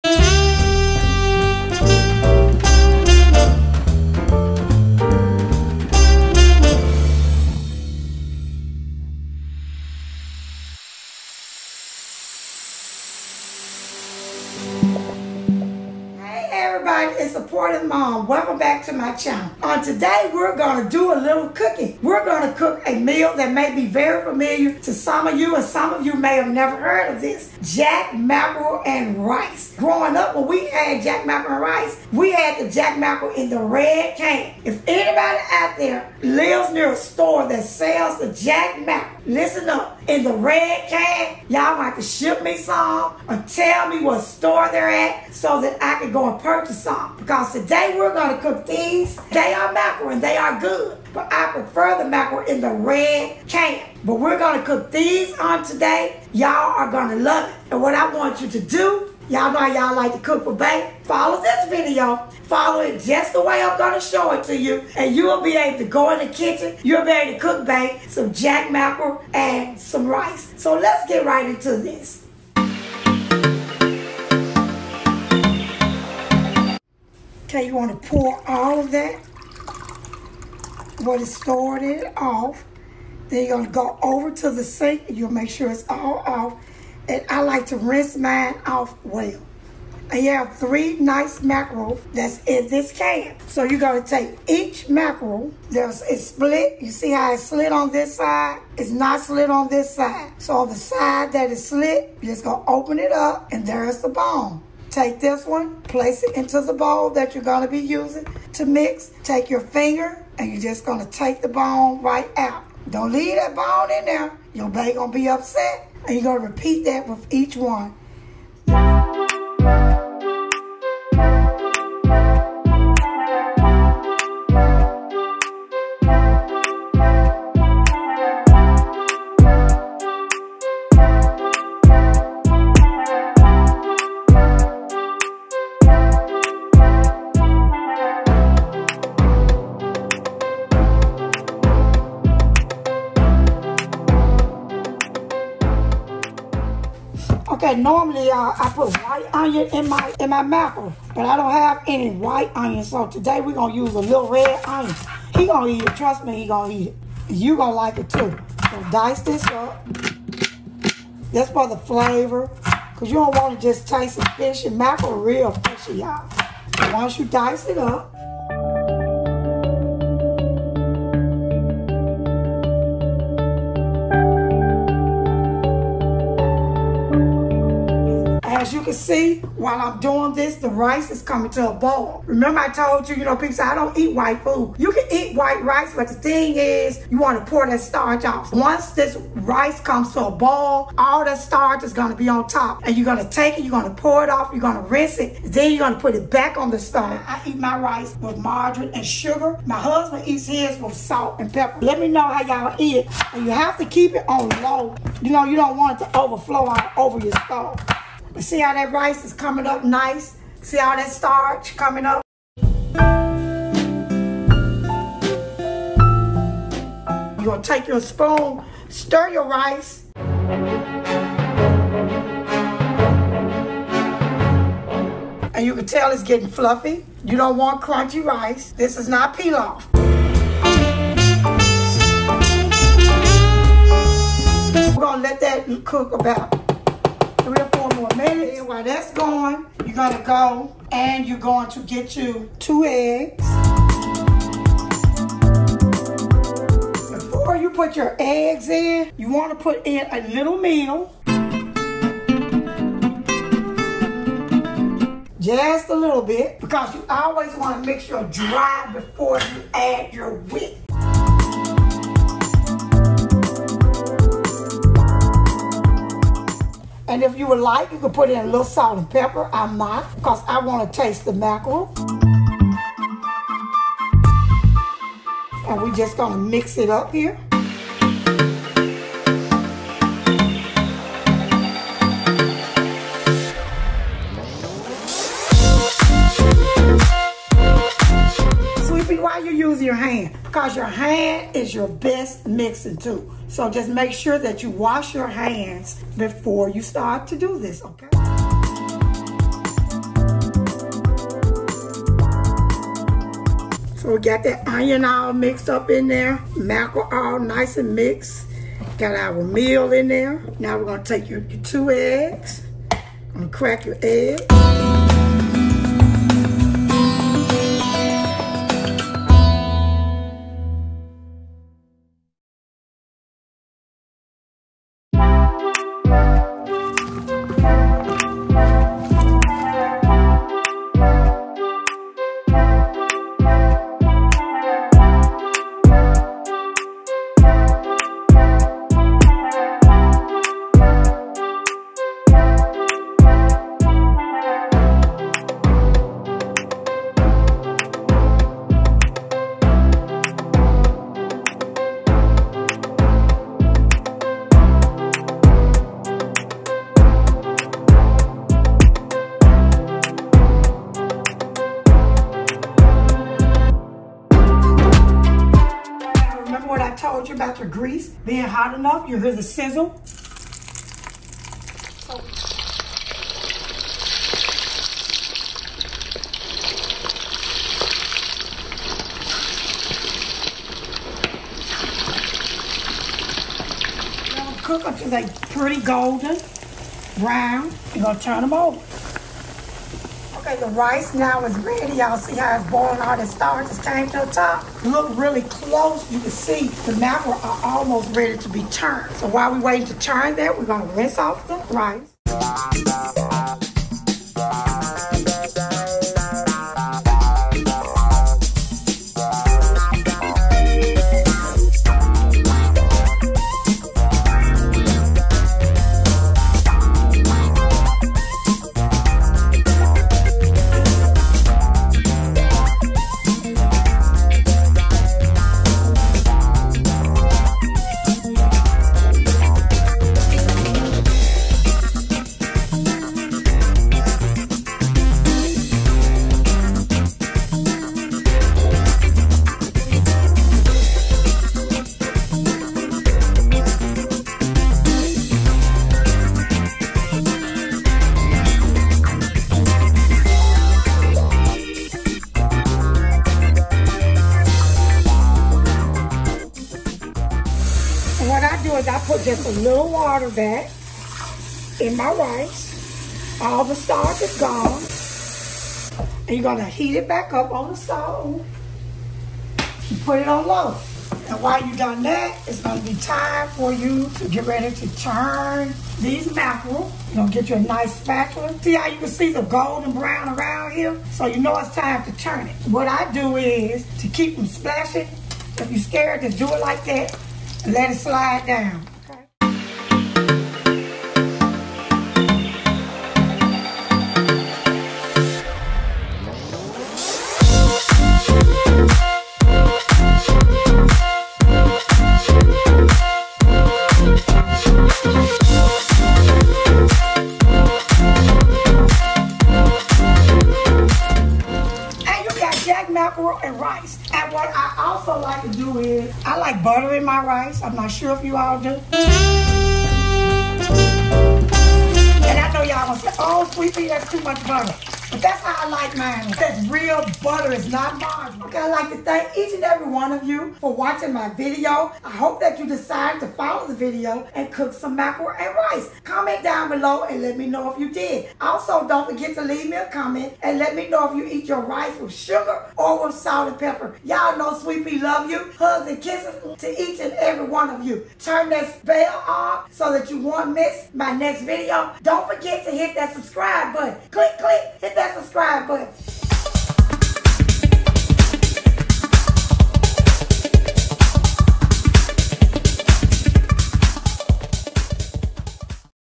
Cook With Me | JACK MACKEREL & RICE
In this video I am cooking  a southern classic meal that so many of us ate as children . Jack Mackerel and Rice.  I show you how to rinse the starch off and enjoy a nice portion of white rice.   I put a lil red onion in the mackerel for flavor and nothing else. I also mention the importance  of thoroughly mixing your dry ingredients before adding the wet ingredients to it so that you know it is mixed throughout well. You may choose to shape them as you please and also make them as large or small as you desire. I hope when you try this recipe you enjoy it . Thanks for cooking with me.

rice is @ricelandfoods3808
ingredients used are: rice, red onion, mackerel, butter, Wesson cooking oil